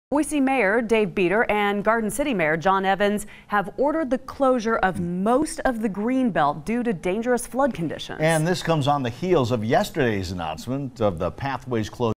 Most of greenbelt now closed in Ada County
Boise Mayor David Bieter and Garden City Mayor John Evans order the closure of most of the greenbelt due to dangerous flood conditions.